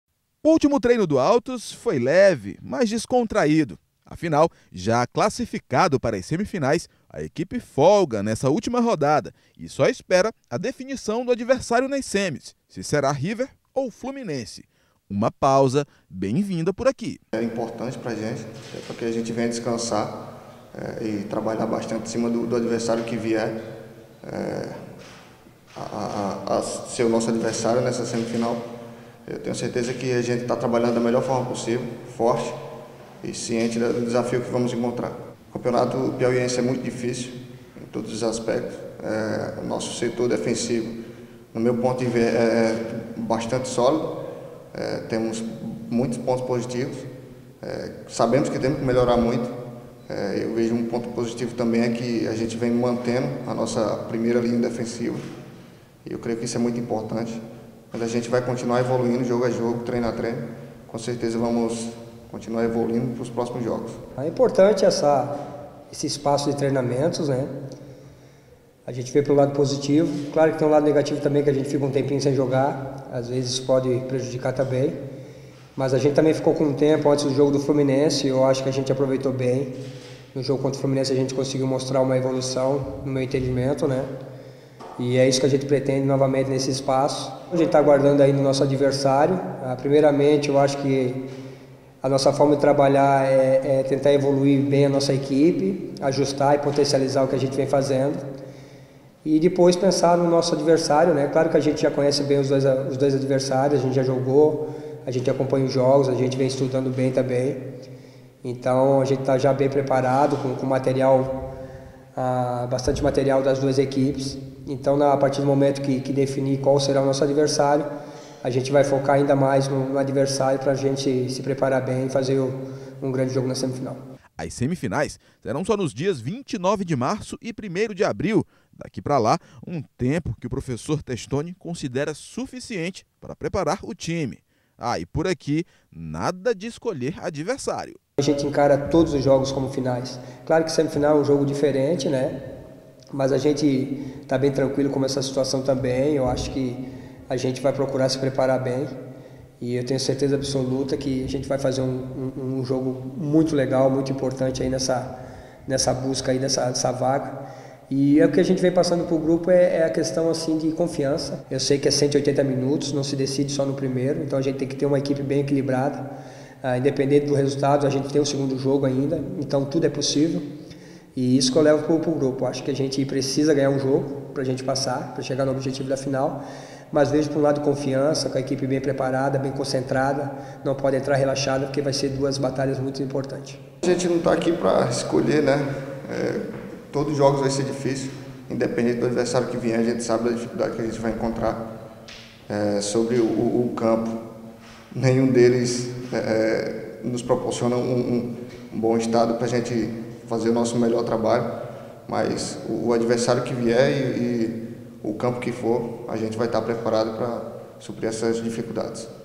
Equipe do Altos espera adversário na próxima fase
O Altos realizou treinou nesta quarta-feira (22). Na equipe do Jacaré, tanto elenco como comissão técnica estão esperando a definição do adversário na próxima fase. Mais detalhes na reportagem de André dos Santos e Volmar Cardoso, da TV Assembleia.